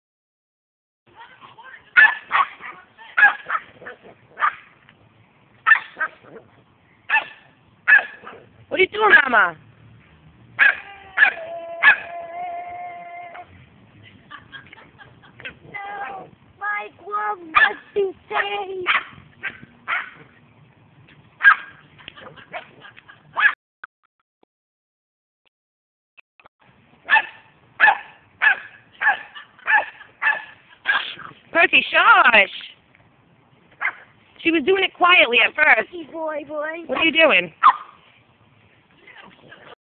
Dog attacking sprinkler
My dog percy, attacking the sprinkler